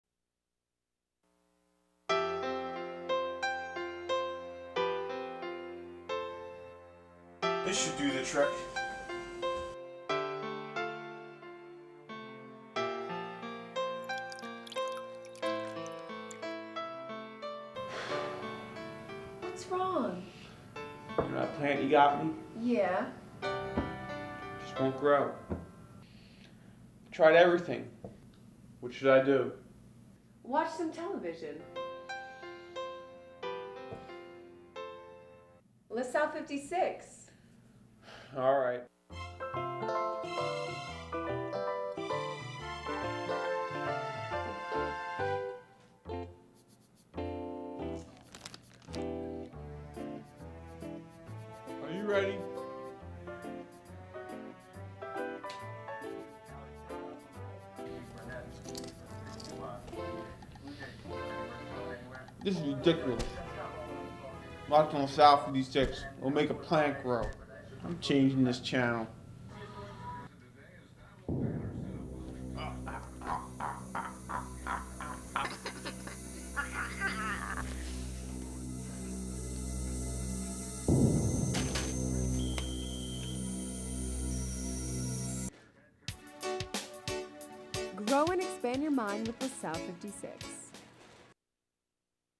Station ID - Come Grow with Us
This ID was created as part of a media production class.